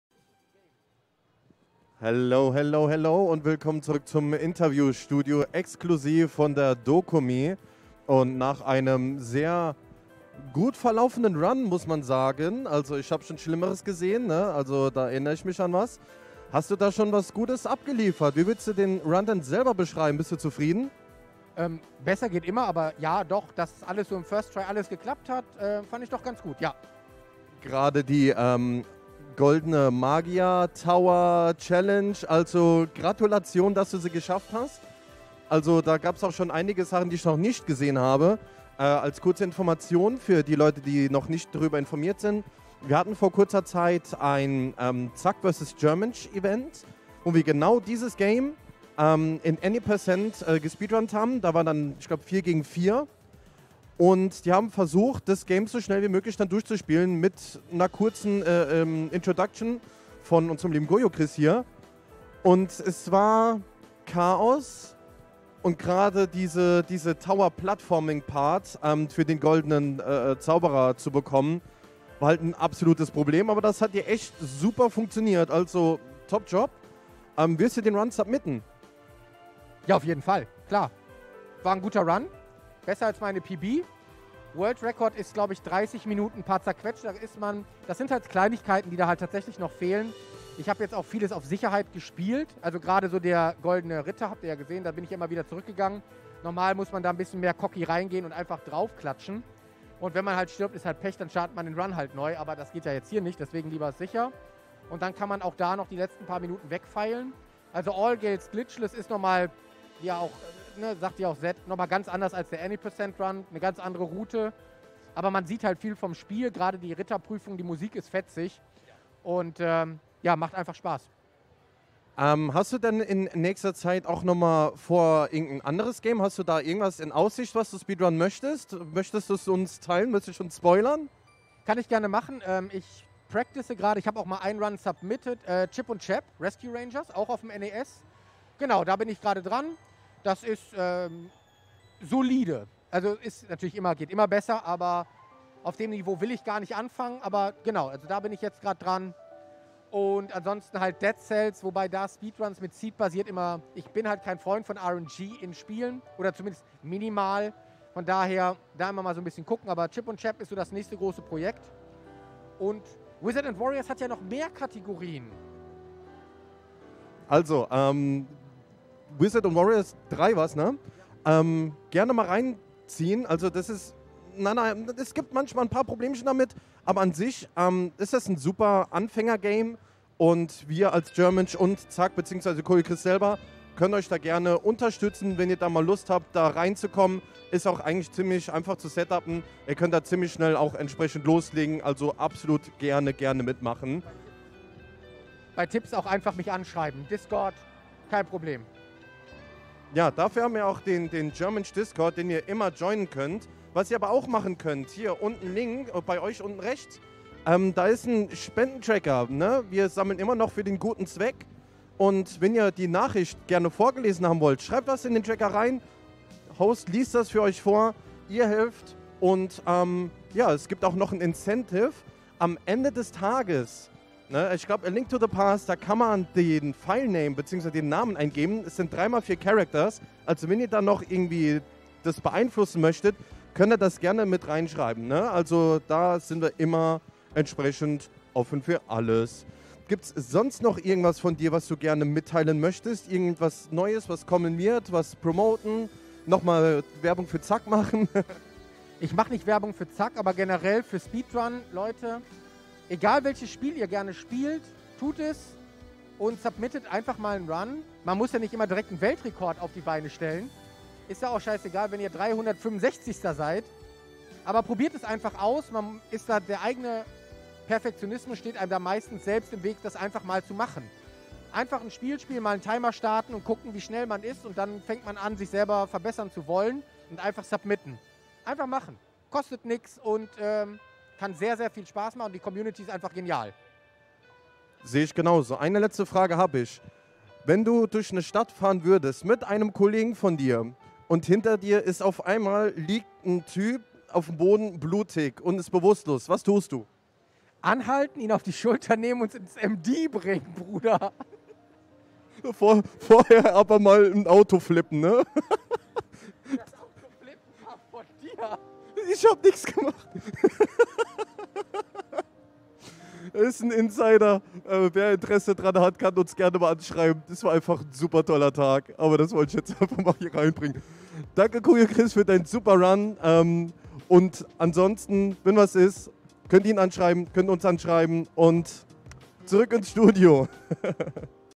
Germench auf der DoKomi 2022: Das Interview von Coyochris mit Fliggmode
Veröffentlicht und präsentiert von Germench - deiner deutschen Speedrun Community.